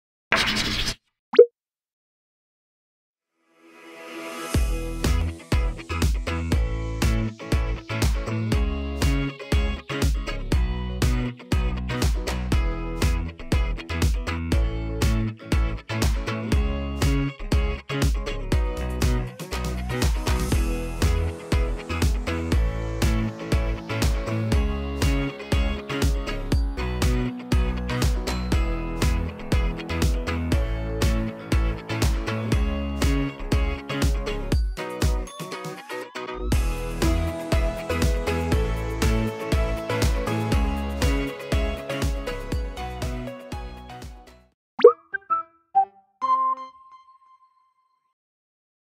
Duplik de Asmodee en Jugaia
Dibuja "una vaca apoyada en 2 esquís que coge un trapecio tirado por una abeja gigante".¡¡¡¡¡¿Qué?!

Y hazlo rápido… No importará lo bien que lo dibujes sino que contenga los elementos importante.